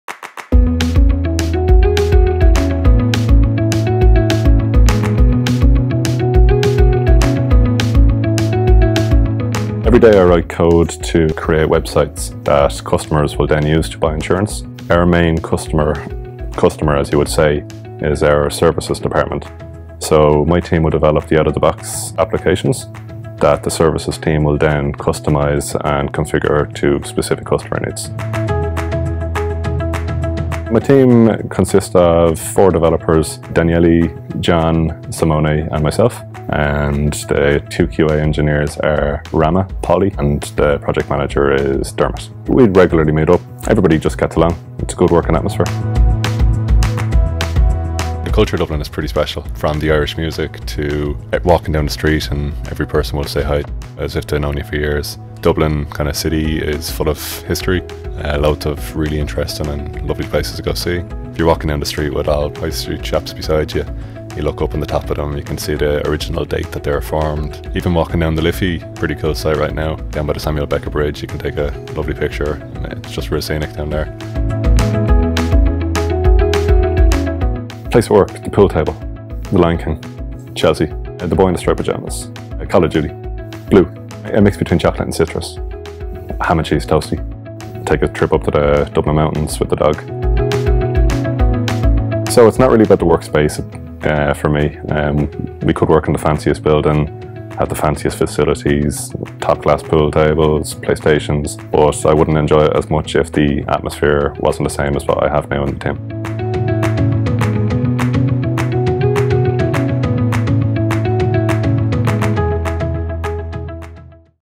My Guidewire | Introducing Gareth
"The culture of Dublin is pretty special."

Senior software engineer, tech lead, Gareth, shared how he got started in Guidewire.

Learn more about how you can be a part of Guidewire

Together, let's navigate what's next.